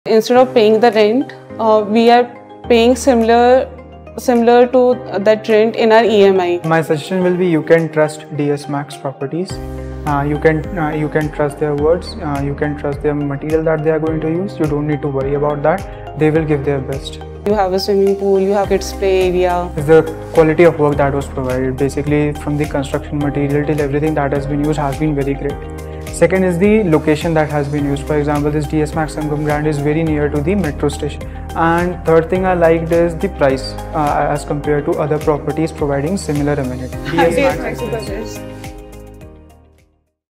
Live Rent-Free in Bangalore's Thriving Hubs with DS-MAX Properties
Imagine living rent-free in your dream Bangalore home. At DS-MAX Properties, that dream becomes reality. Ditch the rent cycle and step into the unparalleled luxury of DS-Max Homes.

We redefine modern living with exquisite quality, cutting-edge amenities, and meticulous design, offering the ultimate comfort and sophistication.

Our apartments are nestled in vibrant neighbourhoods with thriving social scenes and promising futures. DS-MAX properties offer the perfect blend of location and lifestyle. Find the home that embodies your aspirations, where satisfaction and value go hand in hand.

Live rent-free with DS-MAX and transform your Bangalore dreams into reality.